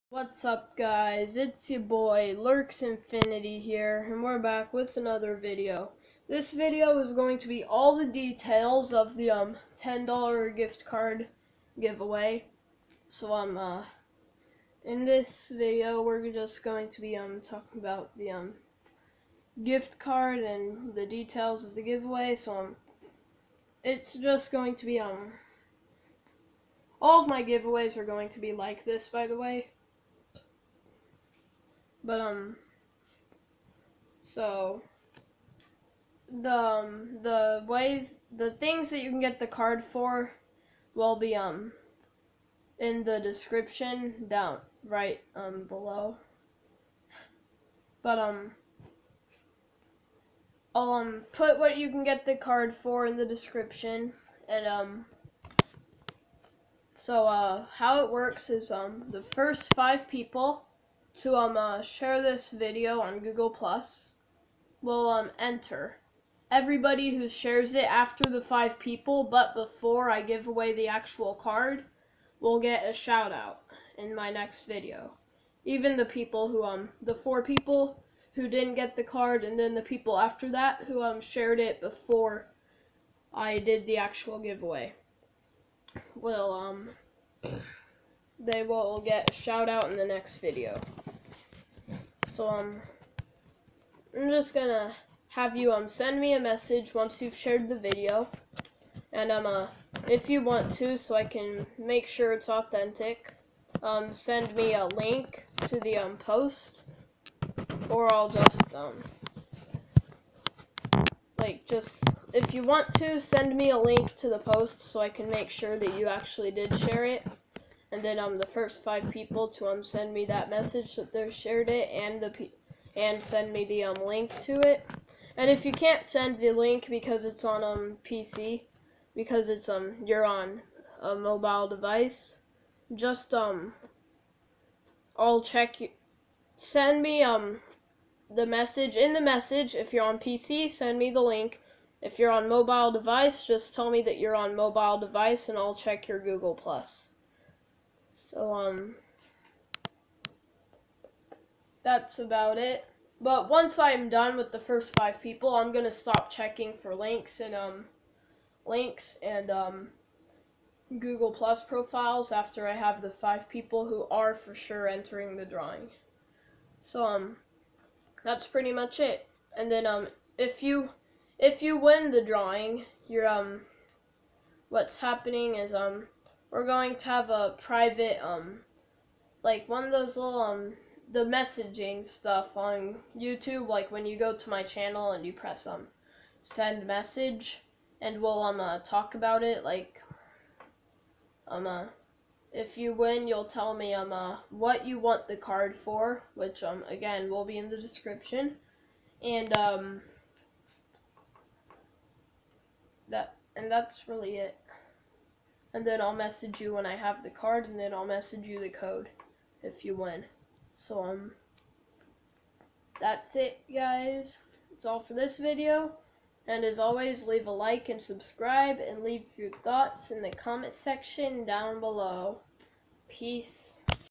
Giveaway Anouncment!!
You can get the $10 gift card for Xbox Live, PSN, Google Play, or iTunes.